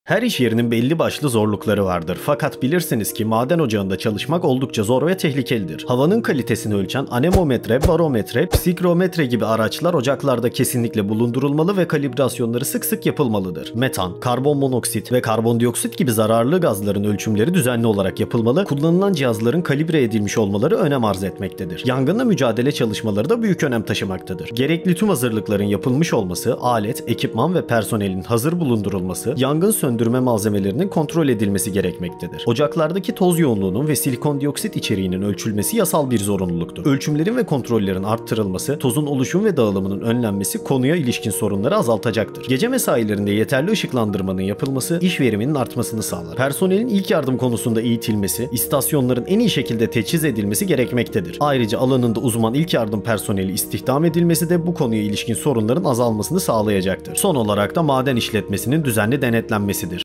Maden Faciası Nasıl Önlenir? Maden Faciası nedir?
Her işyerinin belli başlı zorlukları vardır fakat bilirsiniz ki, maden ocağında çalışmak oldukça zor ve tehlikelidir.
Yeraltı madenciliğında görülen sorunlar şöyle sıralanabilir.
Havanın kalitesini ölçen , Anemometre, Barometre ve Psikrometre gibi araçlar ocaklarda kesinlikle bulundurulmalı ve kalibrasyonları sık sık yapılmalıdır.
Metan , Karbonmonoksit ve Karbondioksit gibi zararlı gazların ölçümleri düzenli olarak yapılmalı, kullanılan cihazların kalibre edilmiş olmaları önem arzetmektedir .
Yangınla mücadele çalışmaları da büyük önem taşımaktadır.
Gerekli tüm hazırlıkların yapılmış olması, alet, ekipman ve personelin hazır bulundurulması, yangın söndürme malzemeleri düzenli kontrol edilmelidir.
Ocaklarda ortamdaki toz yoğunluğunun ve silikondioksit içeriğinin ölçülmesi yasal bir zorunluluktur.
Ölçümlerin ve kontrollerin artırılması, tozun oluşum ve dağılımının önlenmesi konuya ilişkin sorunları azaltacaktır.
Gürültü konusundaysa düzenli olarak gürültü ölçümleri yapılmalıdır.                                                                                                        İzin verilen limitlerin aşılması halinde gerekli önlemlerin alınması, var olan sorunların giderilmesini sağlayacaktır.
Gece mesailerinde  yeterli ışıklandırmanın yapılması, kazaların azalmasını ve  iş veriminin artmasını sağlar. Bu nedenle üzerinde durulması gereken bir konudur.
Personelin ilk yardım konusunda eğitilmesi, istasyonların en iyi şekilde teçhiz edilmesi gerekmektedir, ayrıca  alanında uzman ilk yardım personeli istihdam edilmesi de bu konuya ilişkin sorunların azalmasını sağlayacaktır.
En önemlisi ise maden işletmesinin düzenli denetlenmesi gerekmektedir.

Kaynakça
Elma, Y., Aysan, N. (2017) “Zonguldak Kömür Havzasındaki Maden İşçilerinin Çalışma Koşulları ‘Kamu ve Özel Ocak Karşılaştırması’”, Çalışma Ortamı, S: 154
Aysan, M.E., Kablay, S. (2017) “Türkiye’de Özelleştirmeler ve Kömür Madenciliği: Soma’da Maden İşçisi Olmak” , Ünye İİBF Dergisi, C:1, S:1, s. 51-76
Moyer, F.T. (1992) “Madenlerde Güvenlik”, Çalışma Ortamı, S:2, Mayıs, s. 13-15
Kömür Madenciliğinde İş Güvenliği ve İşçiSağlığına İlişkin Sorunların Değerlendirilmesi ve Çözüm Önerileri - Tevfik GÜYAGÜLER ,  Ümit Yasar ÖMER